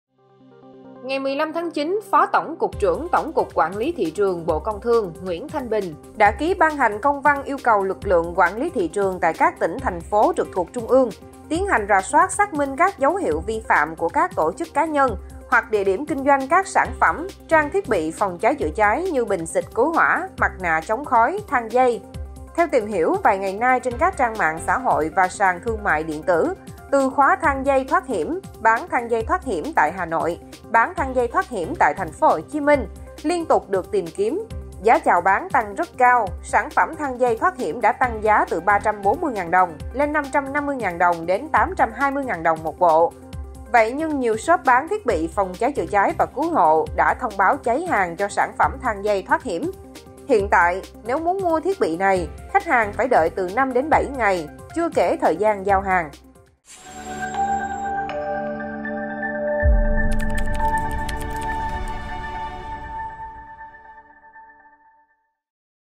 Kiểm tra ngay tình trạng tăng giá thang dây, bình chữa cháy
Ngày 15-9, Phó Tổng Cục trưởng Tổng cục Quản lý thị trường (Bộ Công thương) Nguyễn Thanh Bình đã ký ban hành công văn yêu cầu lực lượng quản lý thị trường tại các tỉnh, thành phố trực thuộc Trung ương tiến hành rà soát, xác minh các dấu hiệu vi phạm của các tổ chức, cá nhân hoặc địa điểm kinh doanh các sản phẩm, trang thiết bị phòng cháy, chữa cháy như bình xịt cứu hỏa, mặt nạ chống khói, thang dây.